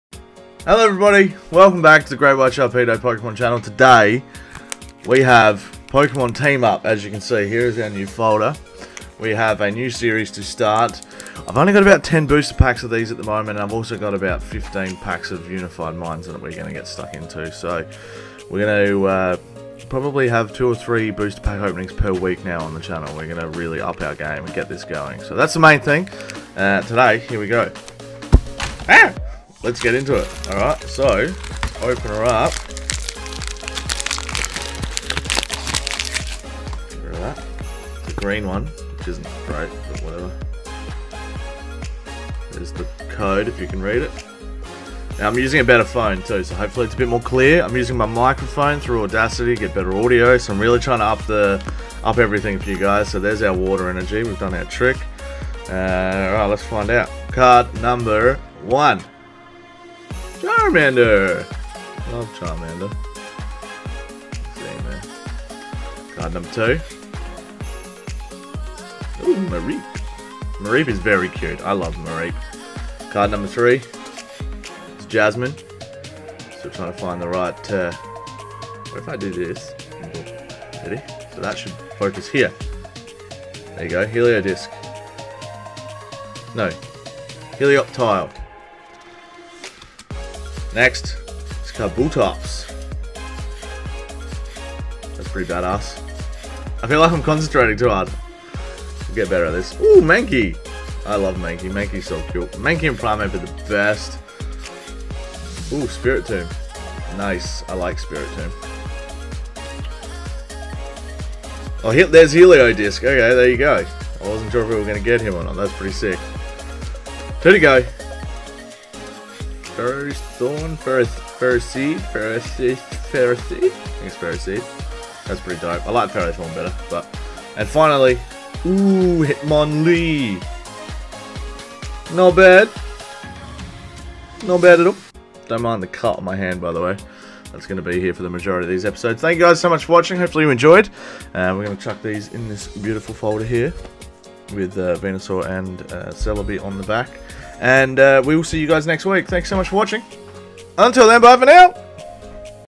Pokemon Team Up TCG Booster Pack Opening. Pack #1
Welcome to Week #1 of my Pokemon TCG (Trading Card Game) Booster Pack Openings Sun and Moon Team Up!
In these videos i will be opening booster packs to try and collect as many rare Pokemon Cards as i can. Specifically the GX cards and the GX TEAM UP Cards. :)